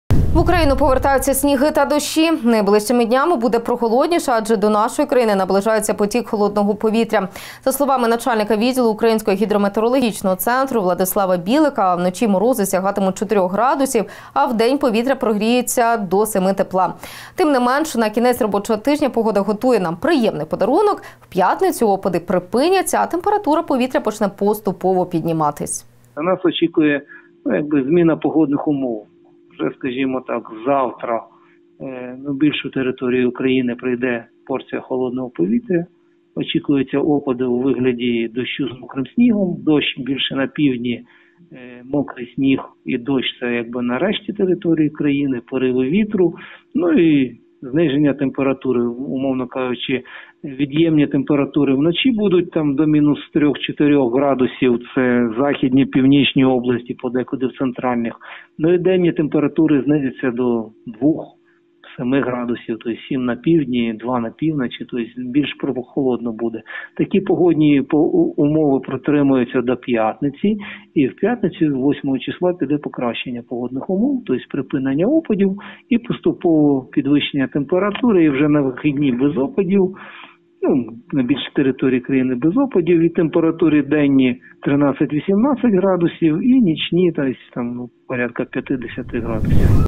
В Україні очікується зміна погод
Від початку весни, погода встигла порадувати нас сонячними днями. Тим не менш, найближчим часом, в Україну повернуться сніги та дощі. На вулицях стане трохи прохолодніше, адже на територію нашої країни наближається потік холодного повітря. За словами начальника відділу Українського гідрометеорологічного центру Владислава Білика, вночі морози сягатимуть чотирьох градусів, у в день повітря прогріється до семи градусів тепла. Тим не менш, на кінець робочого тижня, погода готує нам приємний подарунок. У п’ятницю опади припиняться, а температура повітря  почне поступово підійматися.